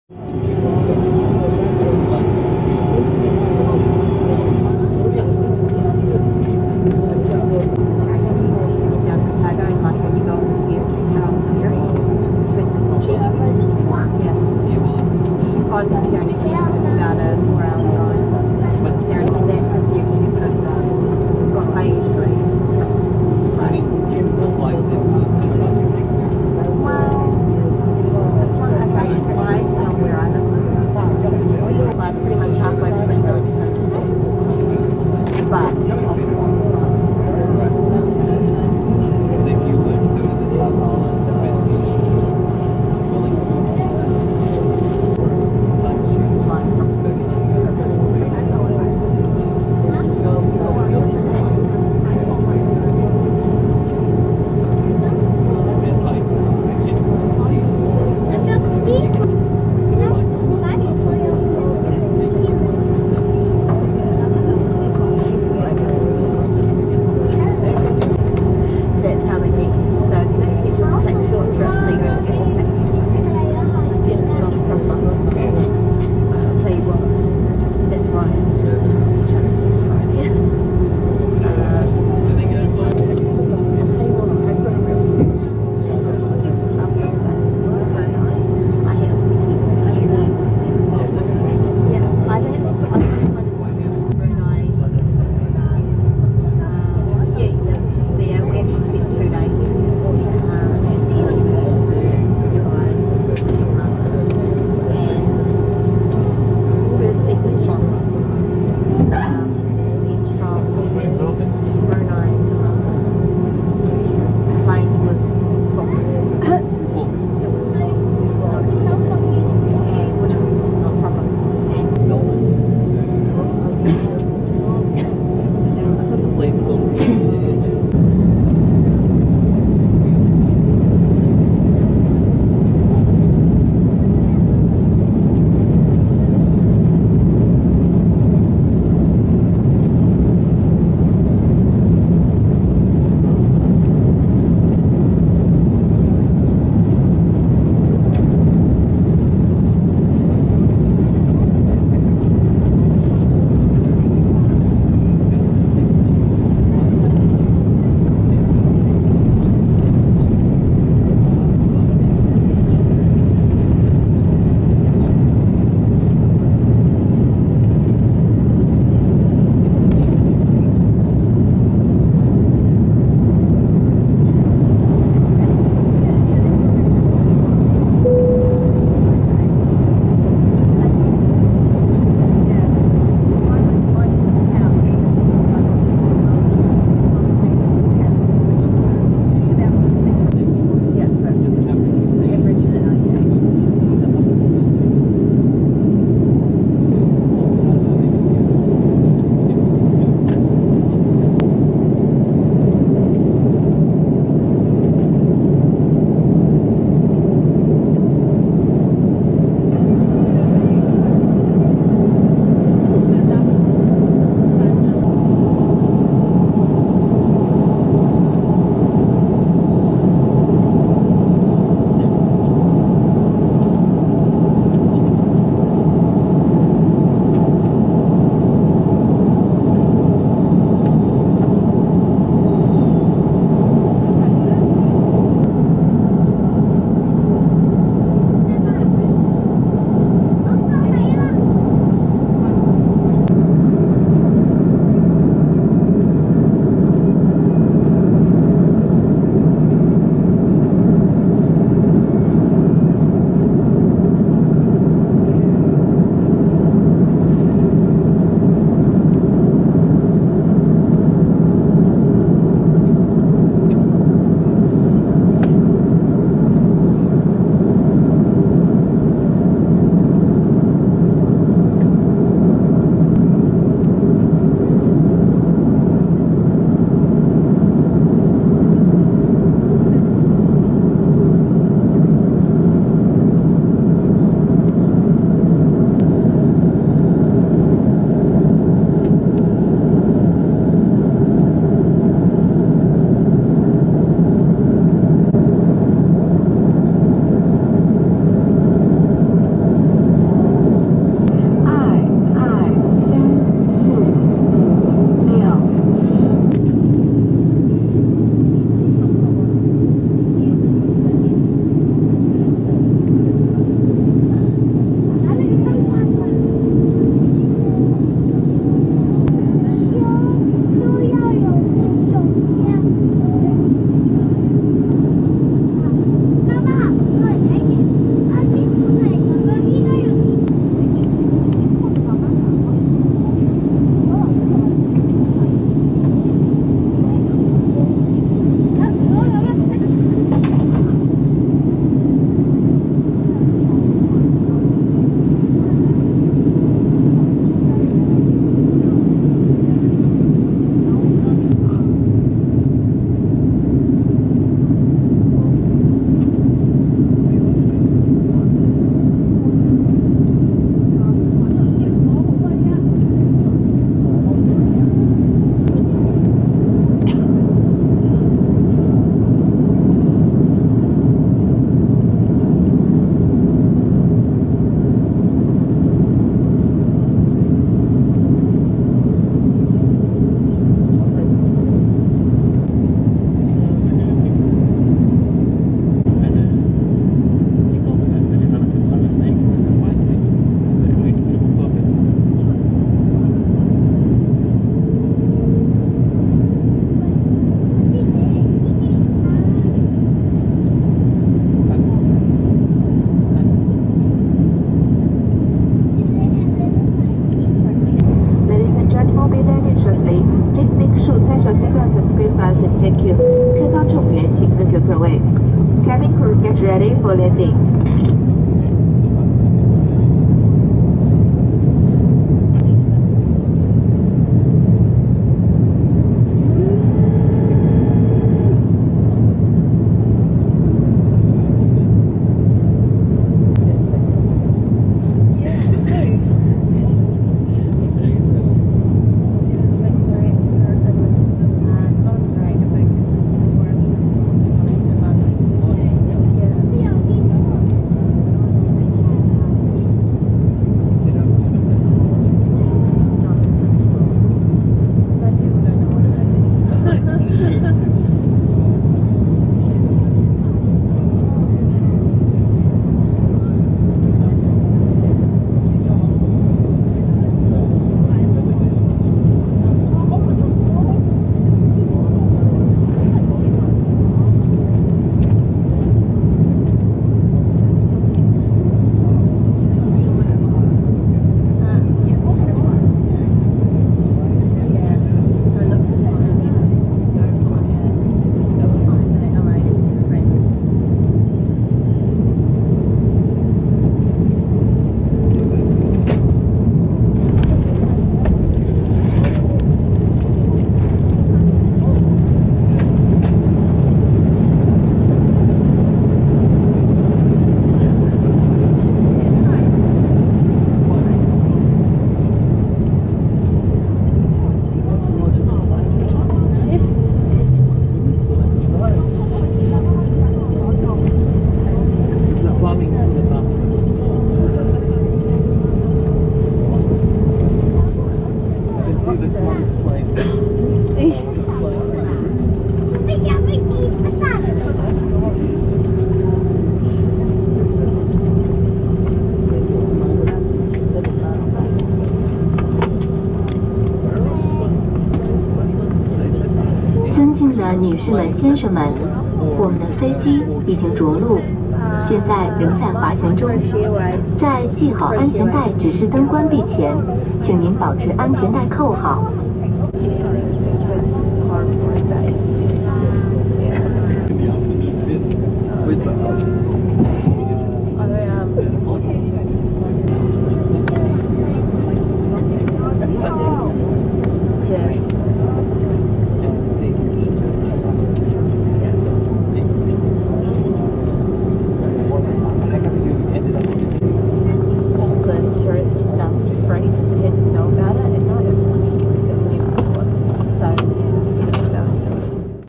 Canton - London CAN-LHR, China Southern A-330, April 2013
Airbus A-330 B-6526. Flight from Guangzhou /Canton/ to London Heathrow, CAN-LHR. Recorded on 25 April 2013